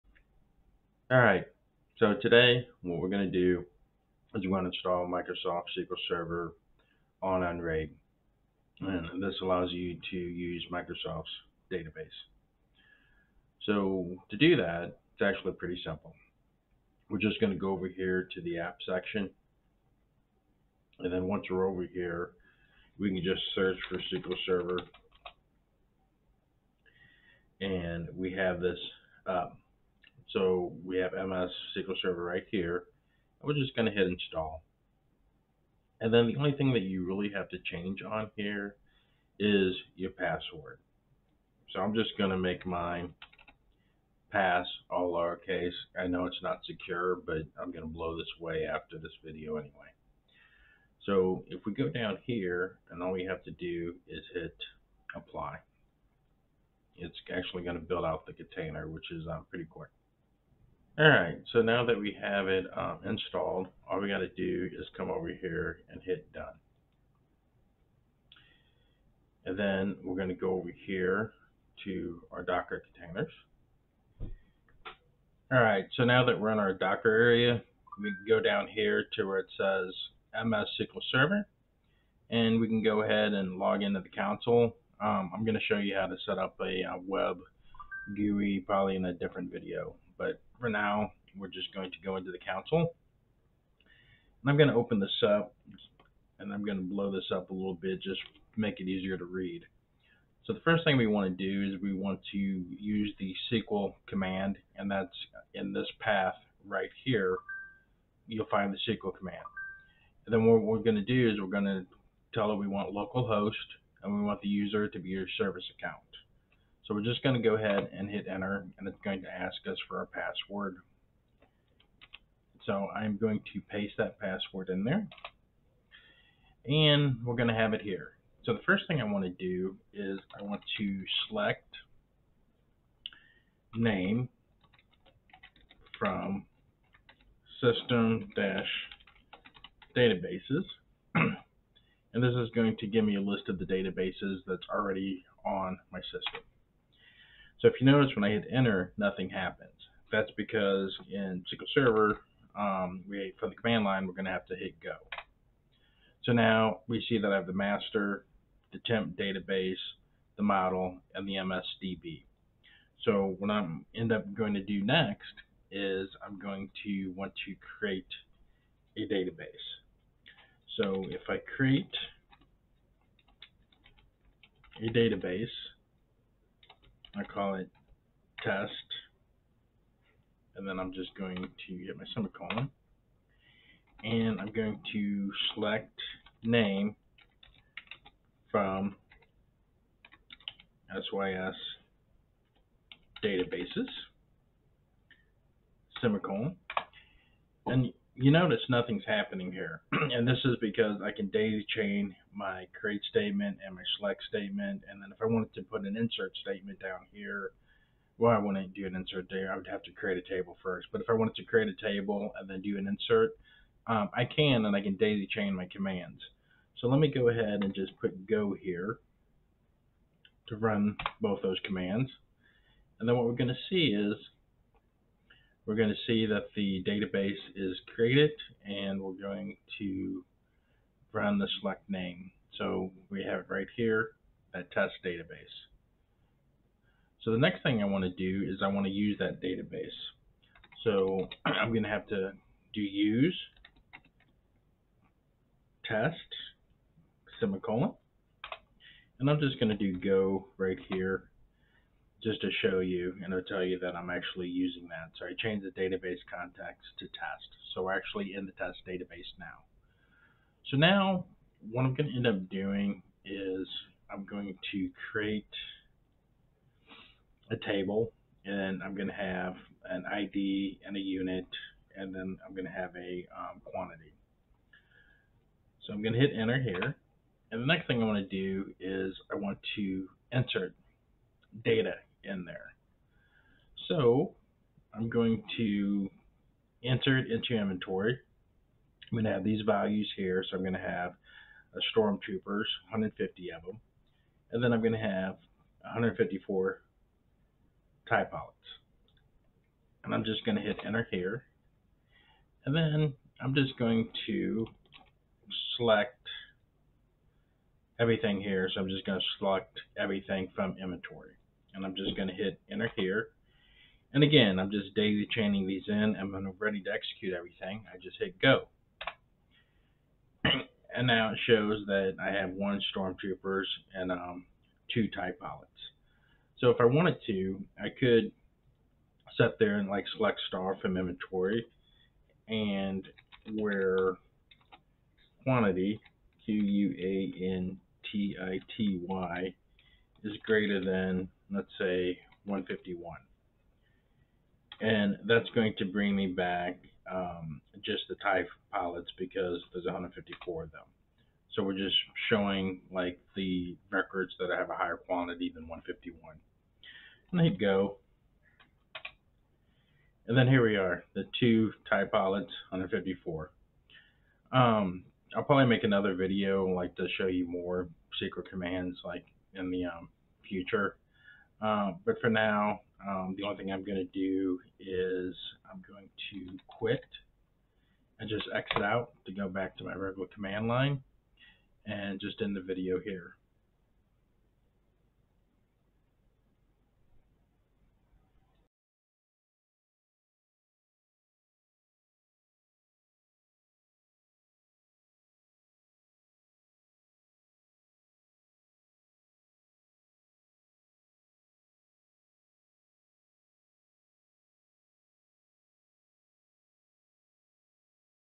SQL Server on UnRaid!
This video will show you how to install and use SQL Server on UnRaid using a docker container from the community store.

CHAPTERS
00:00 Intro
00:16 Go to Apps and Select MS-SQL Server
00:35 Install MS-SQL Server
01:09 Goto Docker and Open Container
01:33 Console Commands to Login
02:08 Basic SQL Commands
06:29 Like and Subscribe